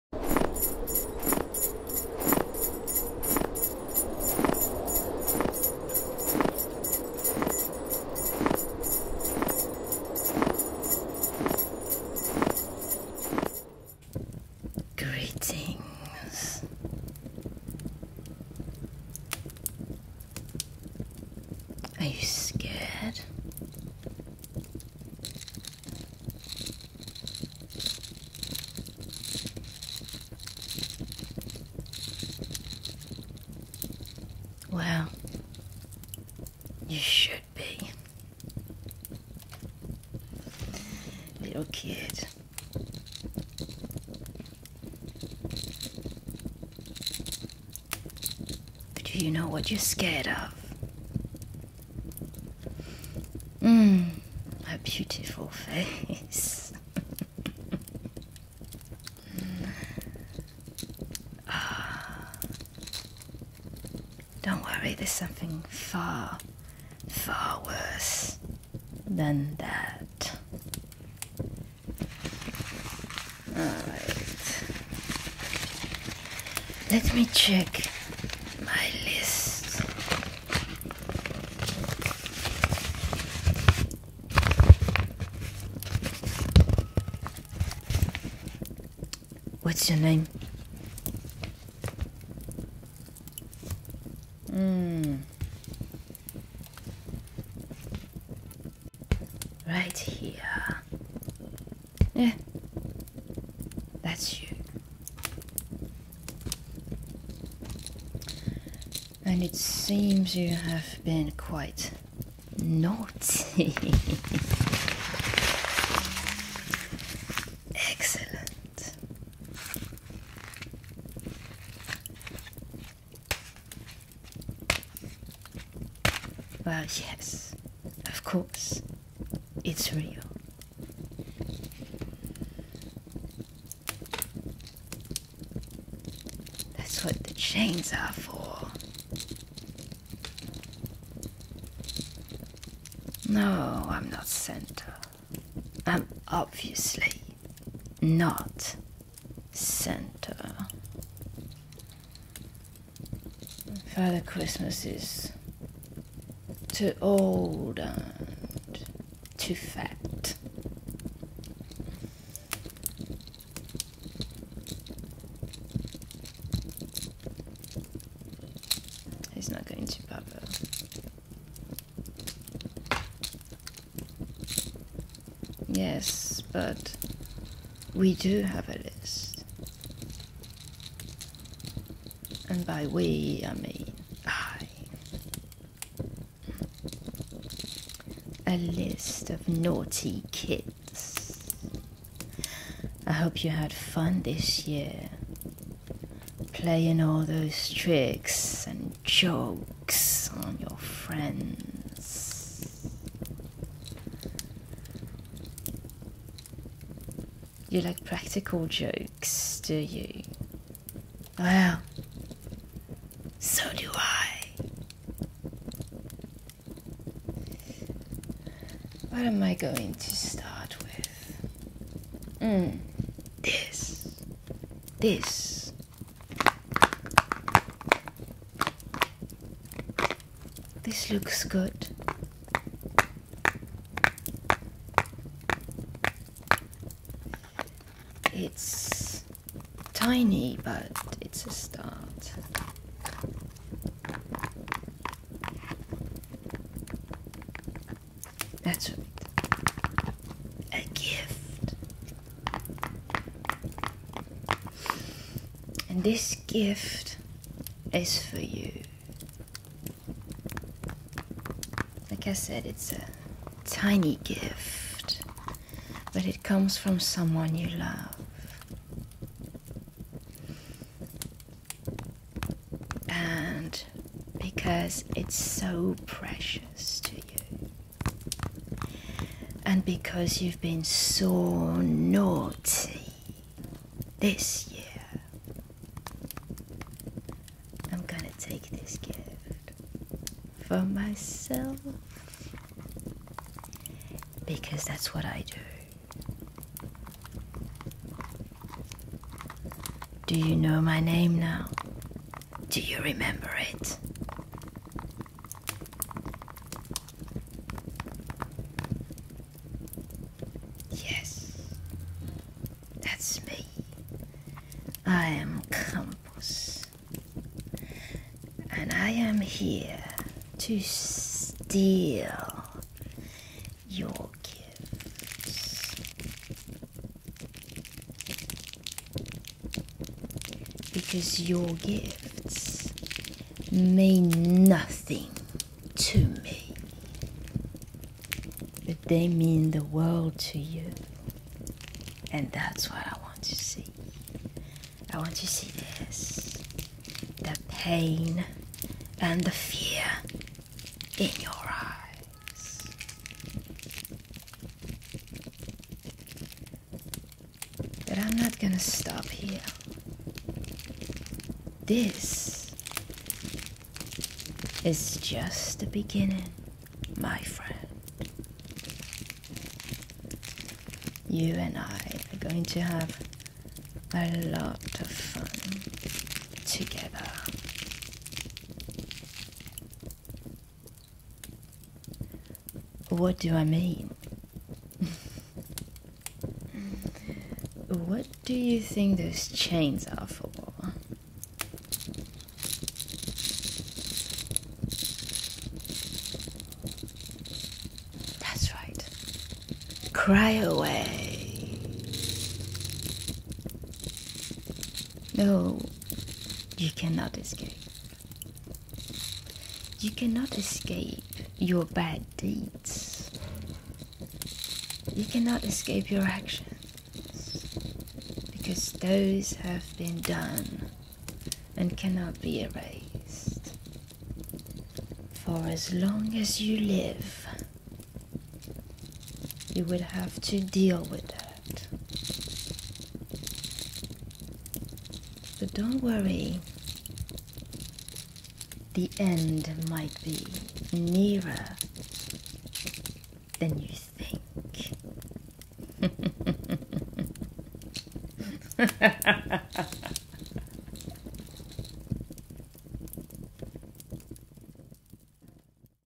KRAMPUS - dark ASMR (fire, clickety & crinkly sounds)
Guess who’s on the naughty list!!

Snow footage, wind blowing, footsteps in snow, bells jingling and fire sounds from pixabay.com.